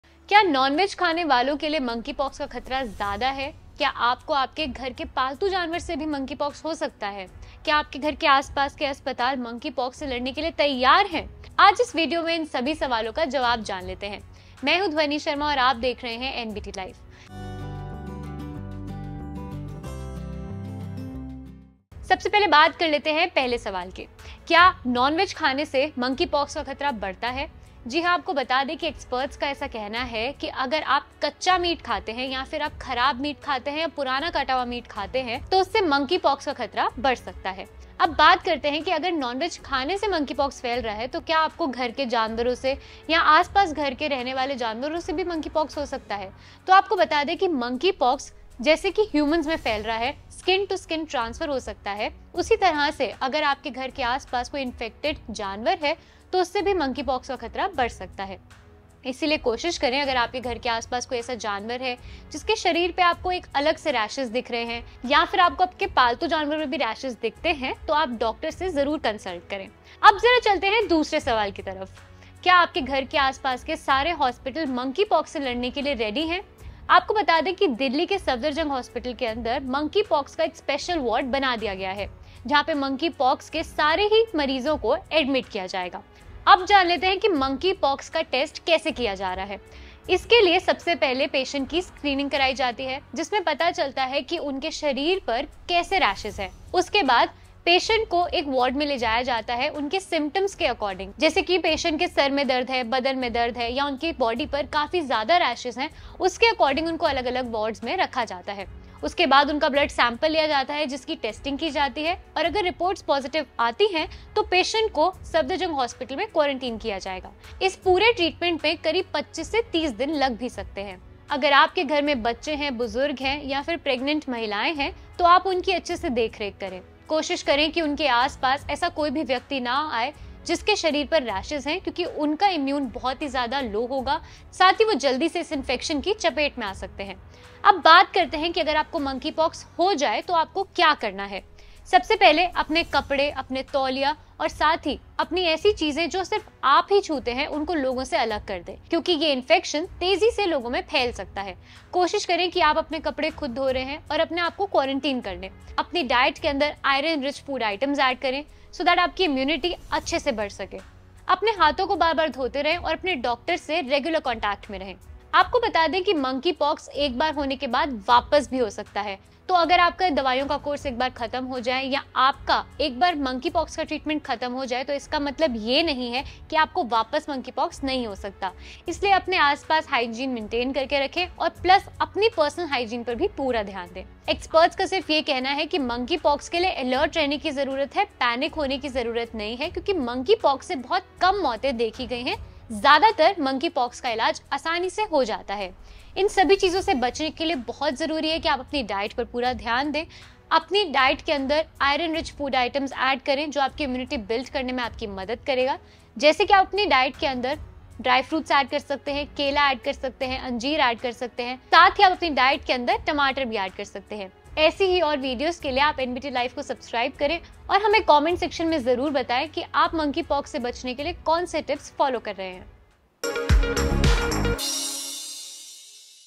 Monkey Pox Virus: क्या Nonveg खाने से हो सकता है मंकीपॉक्स? | Monkeypox Ka Ilaj Kya Hai | NBT Life
Monkeypox virus In India: WHO ने मंकीपॉक्स को सबसे खतरनाक बीमारियों की लिस्ट में शामिल किया है, यह वायरस 17 देशों में फैल चुका है. यह बीमारी खास तौर पर 15 साल से कम उम्र के बच्चों और युवाओं को अपनी चपेट में ले रही है। क्या Nonveg खाने वालों को मंकीपॉक्स से ज्यादा खतरा है?, जानें इस वीडियो में